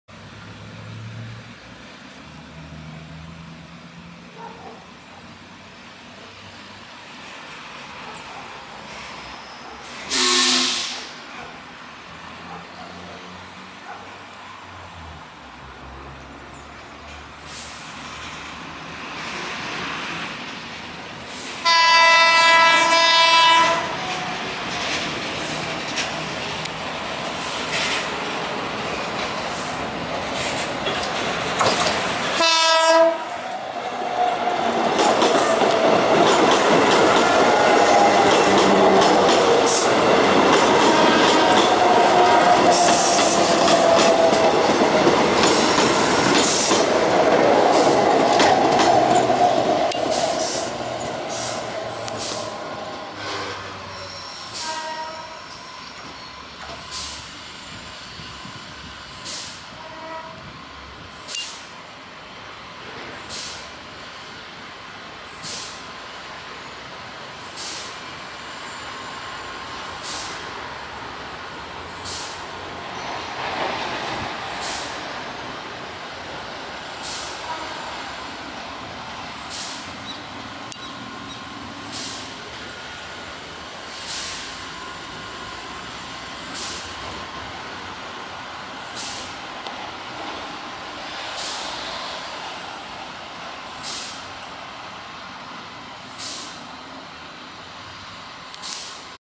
Tren llegando a Martín Coronado desde Pablo Podestá
Ferrocarril Urquiza
15 de mayo de 2015
13:32